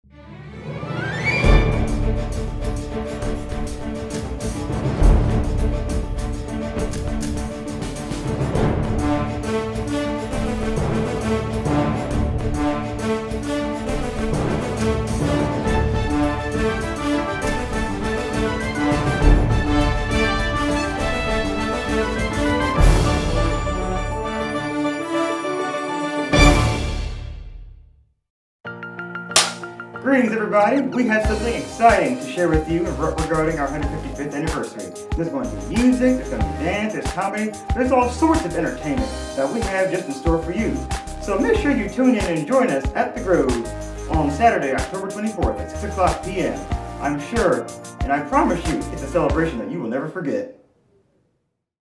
155th Anniversary Promo / Alex Croney
Pleasant Grove Church 155th Anniversary

Theme: Celebrating Favor from Our Faithful God
22 Because of the Lord’s great love we are not consumed, for his compassions never fail.
23 They are new every morning; great is your faithfulness. 24 I say to myself, “The Lord is my
portion: therefore, I will wait for him.”
Lamentations 3:22-24 (NIV)

Saturday, October 24, 2020 at 6:00pm
Celebration and Dedication of The Pleasant Grove Family Life Center
Christian comedian ~ Dance ~ Music

Sunday, October 25, 2020 at 9:45am
Worship Service